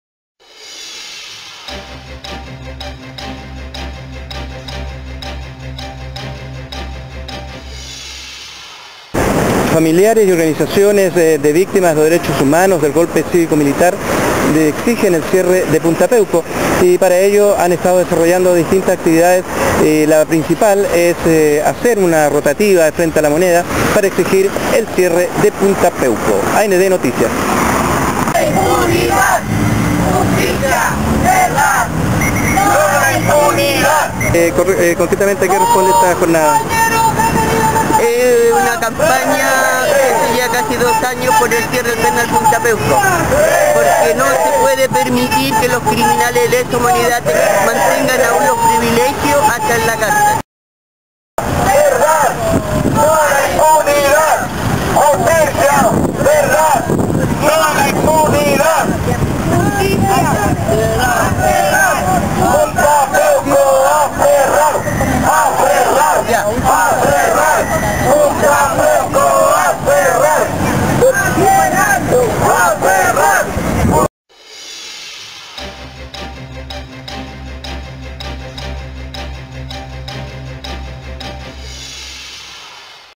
Familiares de victimas de DD HH Piden Cierre de Punta Peuco
Santiago de Chile 10 Diciembre de 2017/ Familiares de ejecutados políticos y victimas de la represión tras el Golpe Civivo -Militar del 11 de septiembre de  1973,  Piden cierre de penal de Punta Peuco y para ello desarrollan todos los días viernes una protesta pacifica frente a La Moneda -señalo Alicia Lira-
AND Noticias